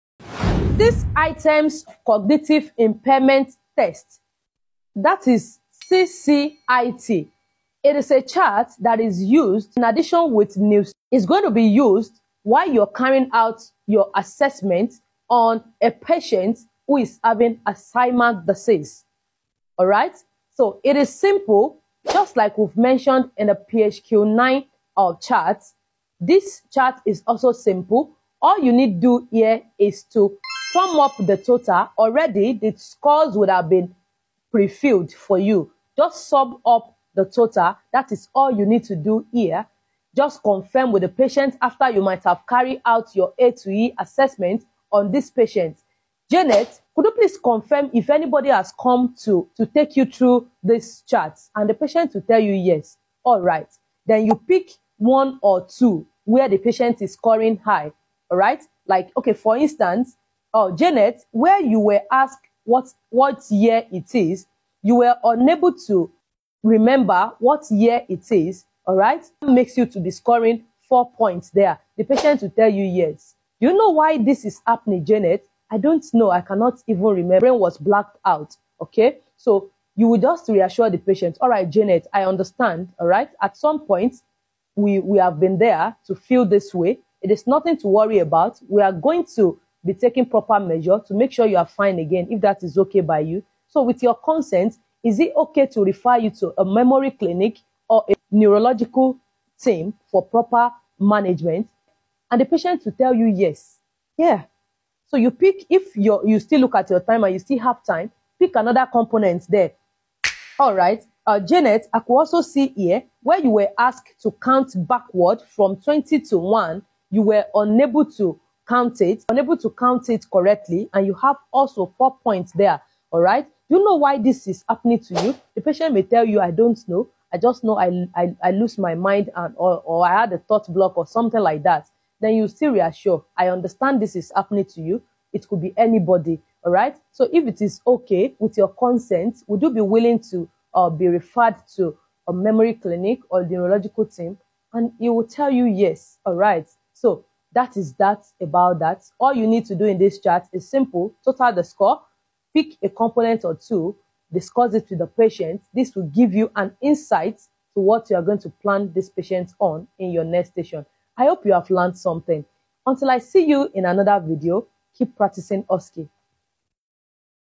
NMC OSCE: Assessment station for an Alzheimer scenario
Watch this to the end to have a deeper understanding about 6CIT CHART FOR AN ALZHEIMER'S PATIENT